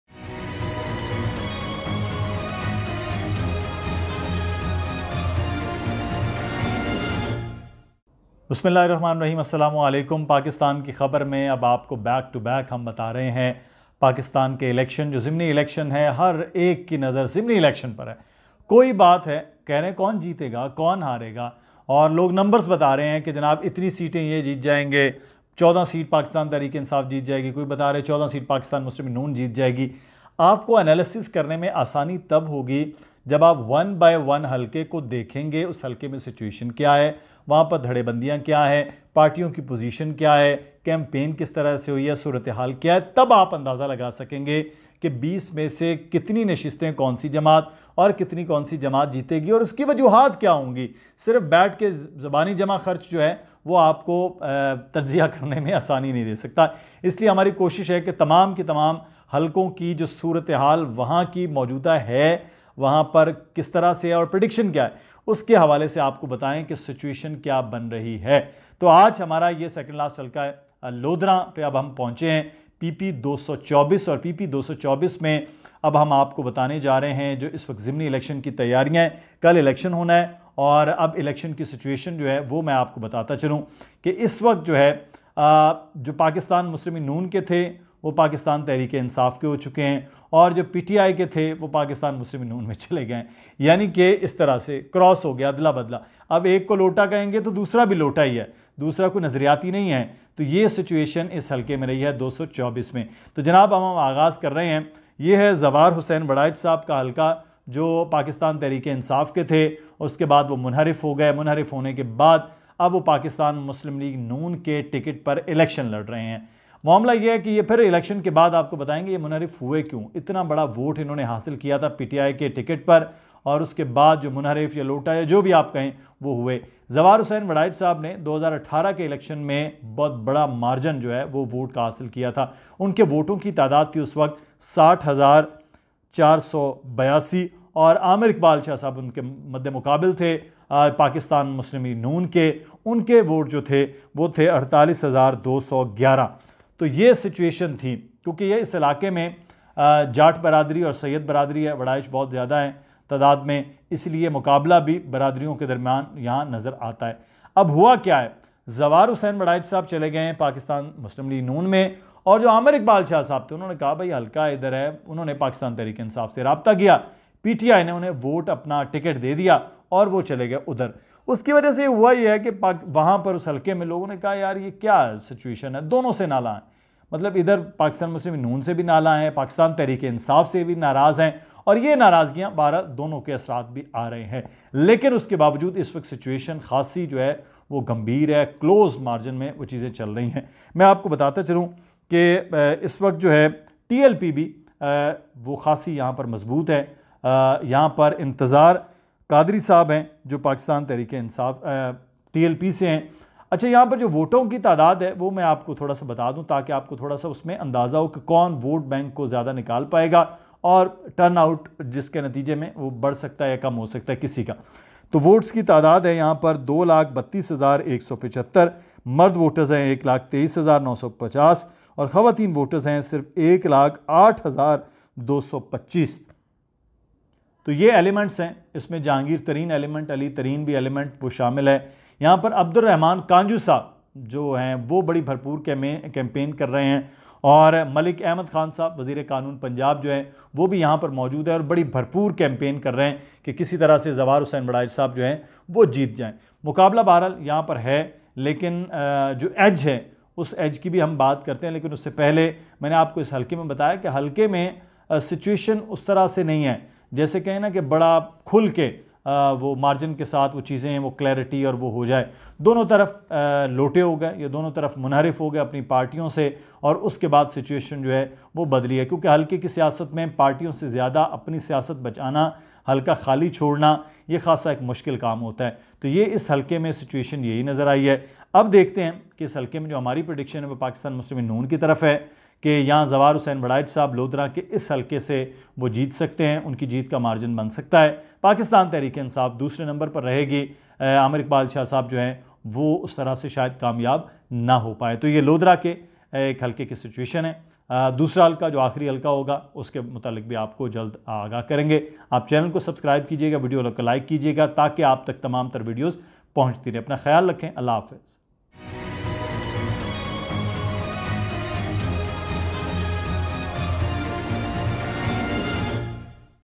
By Election PP 224 Lodhran | Zawar Hussain Warraich vs Pir Amir Iqbal Shah | Jahangir Tareen Factor
Mudasser Iqbal shares the details regarding By Election in pp 224 Lodhran where Zawar Hussain Warraich of PMLN and Pir Amir Iqbal Shah of PTI are Contesting Election.